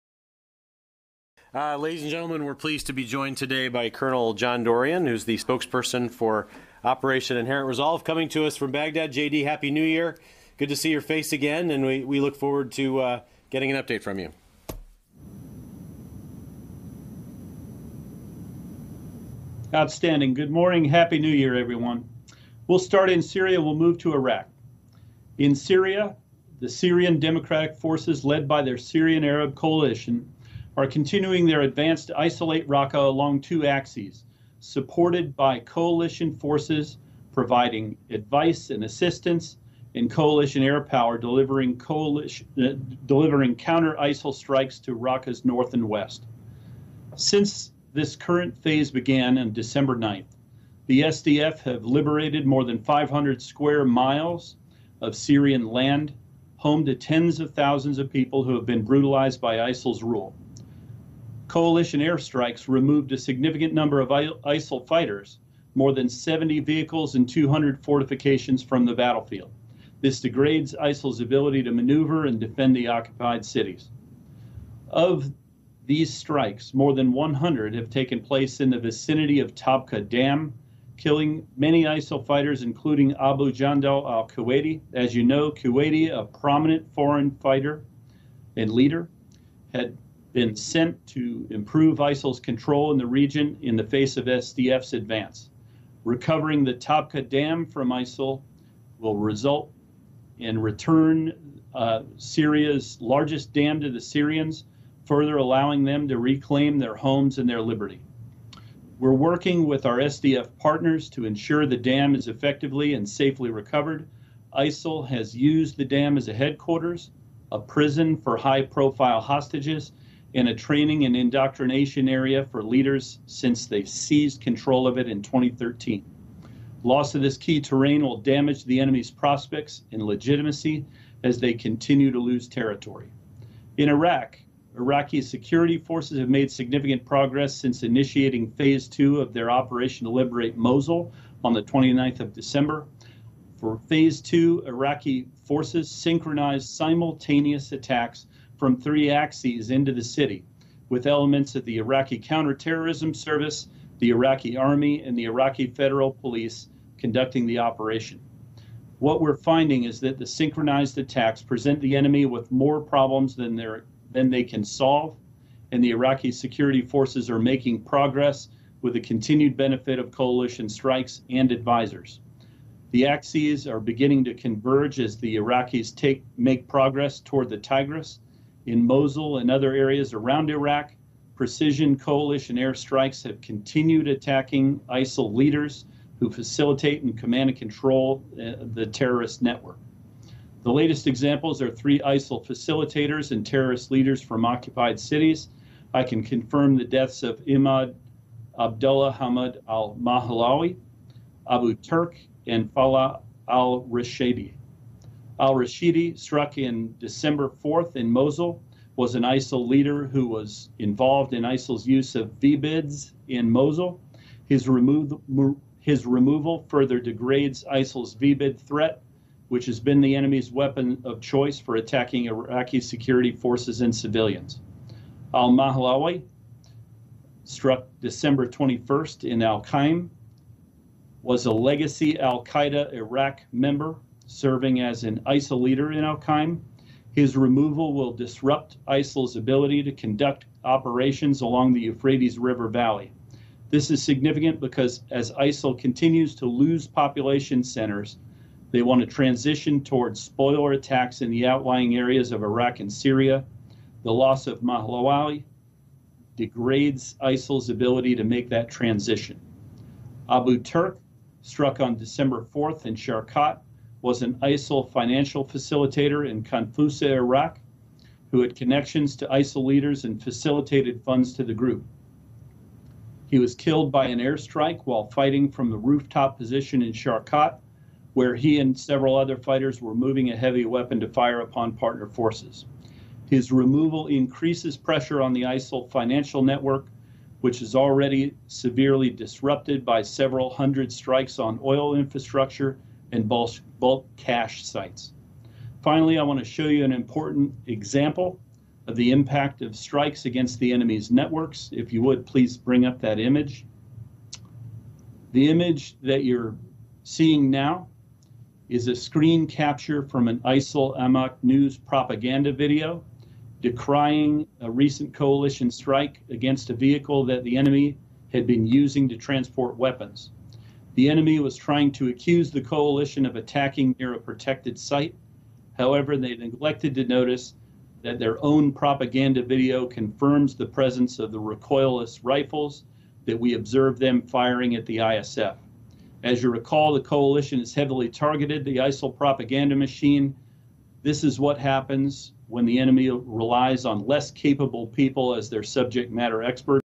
NATO: MOSUL/RAQQA. Col. Dorrian Updates & Answers Press Questions On the Battles.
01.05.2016. Baghdad, Iraq. (Error - uploaded 5 minutes only)
U.S. Air Force Col. John Dorrian provides the pentagon with operations updates, and answers press reporter's questions.